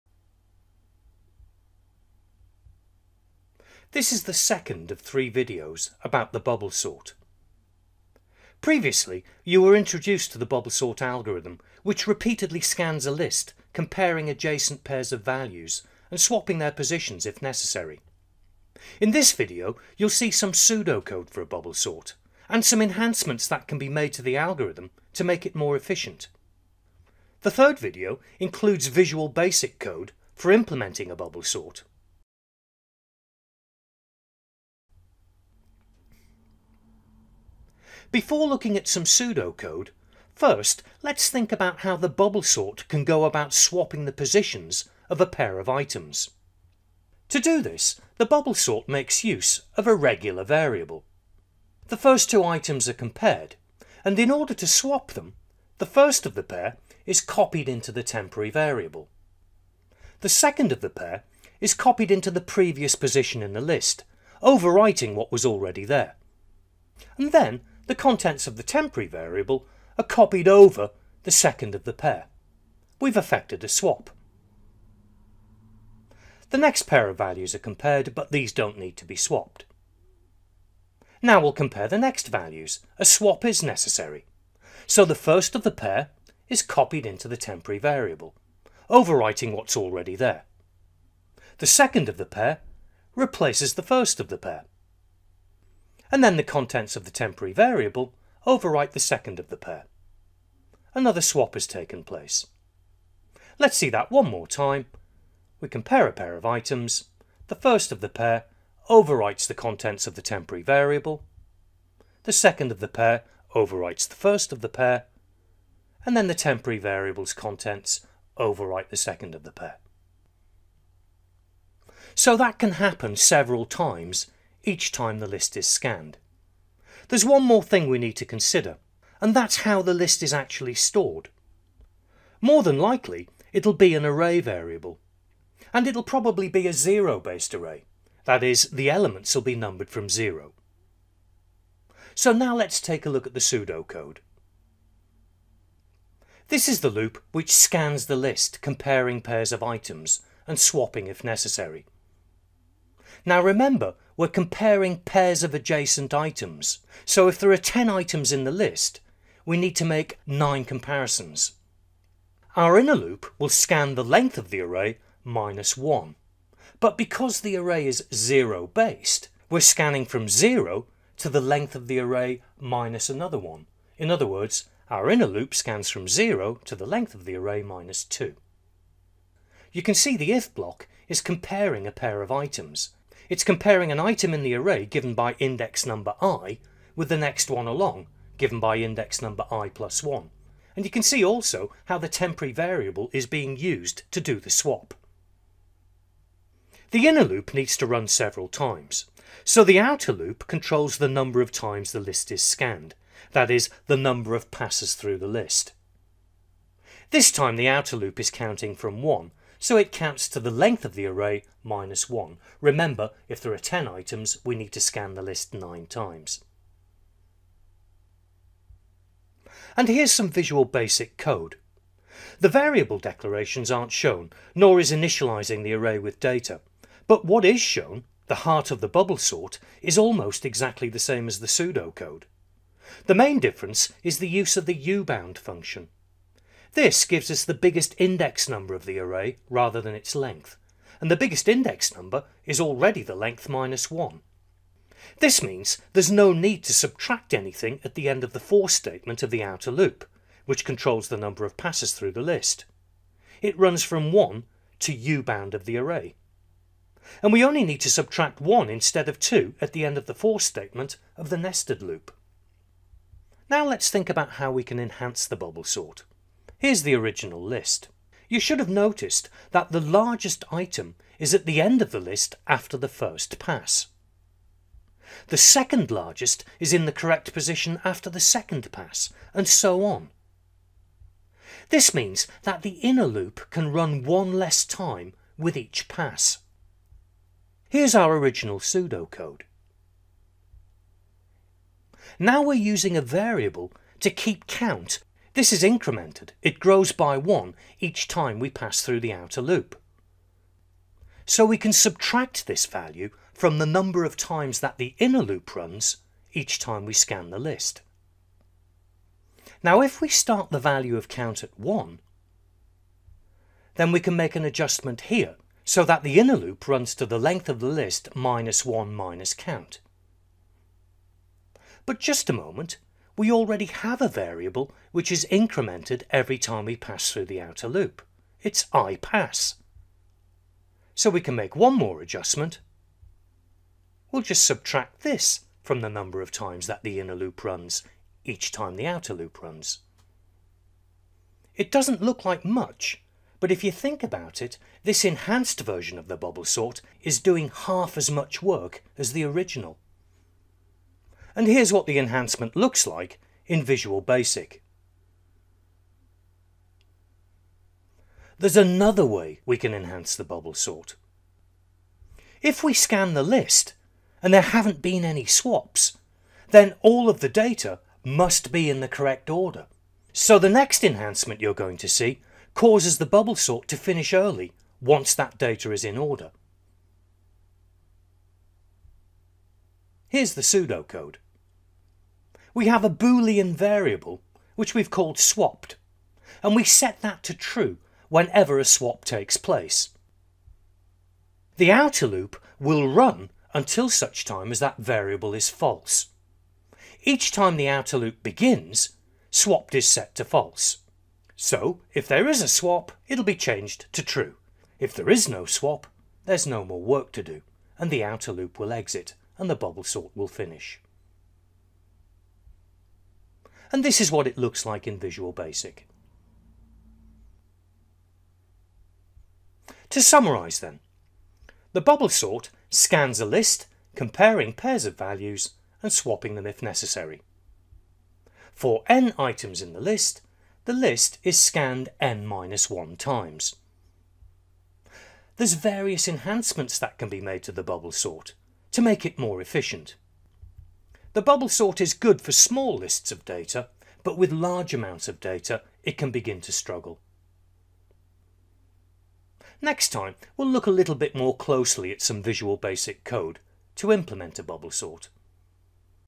Bubble Sort 2- Pseudocode and Enhancements
This is the second of four videos about the bubble sort.  The bubble sort, otherwise known as the ripple sort or the sinking sort is one of the simplest sorting algorithms and is therefore relatively easy to understand and implement.  The bubble sort performs well for relatively small amounts of data, but struggles with very large amounts of data.  This video describes pseudocode for a simple bubble sort, and for some enhancements.  The videos that follow include VB.NET implementation code for a bubble sort.